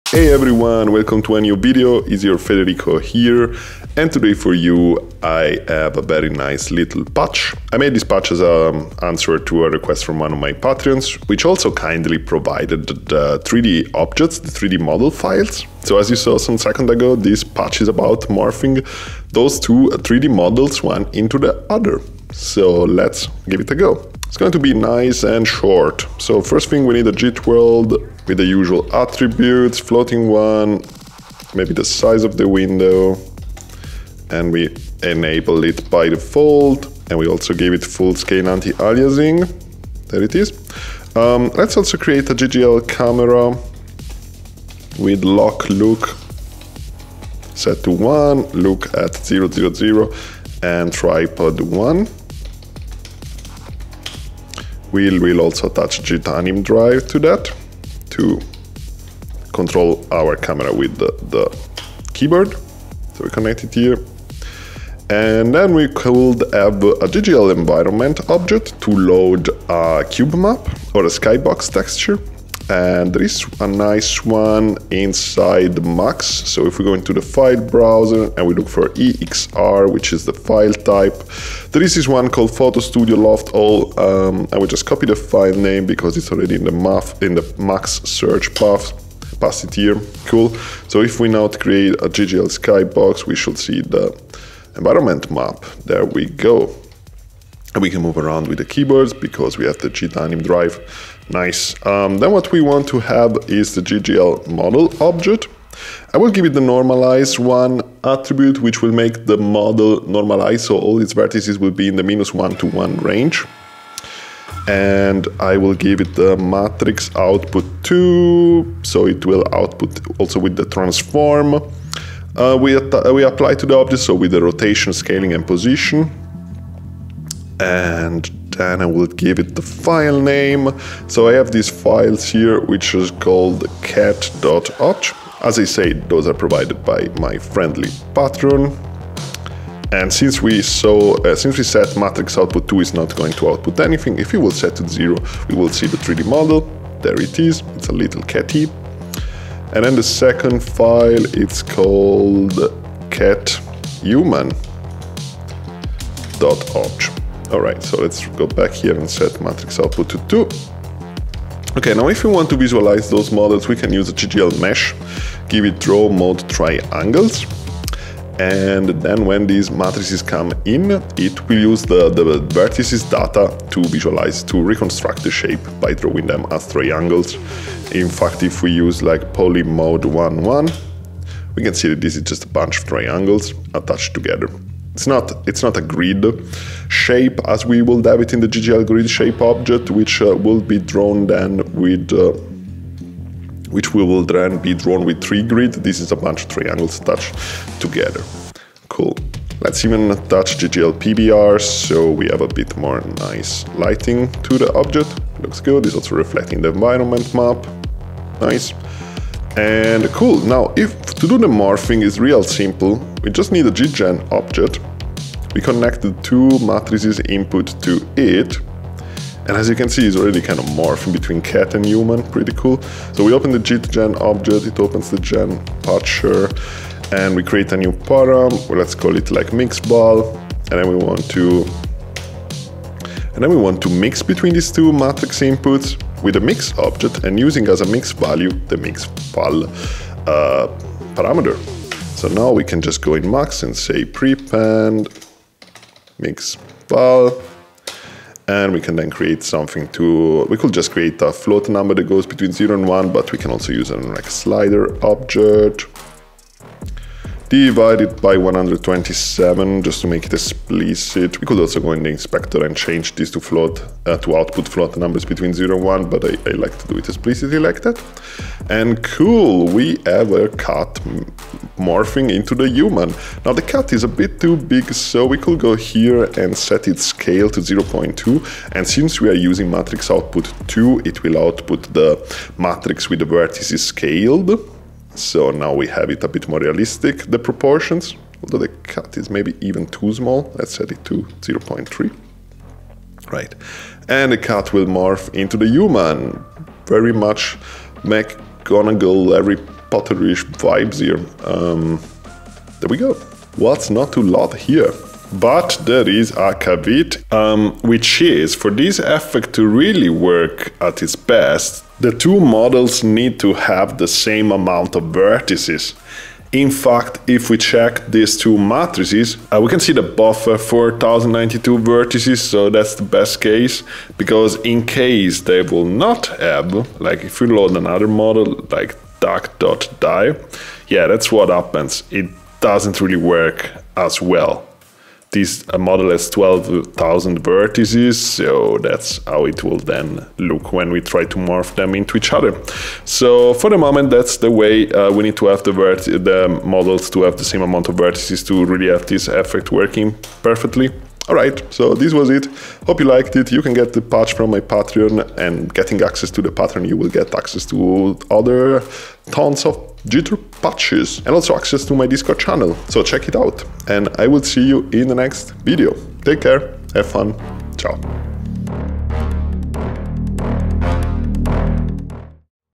Morph from one 3D Model to Another with Max/MSP Jitter
In this video we see how to linearly mix two 3D models in Max.
It's important to note that for this effect to work the two models need to have the same amount of vertices.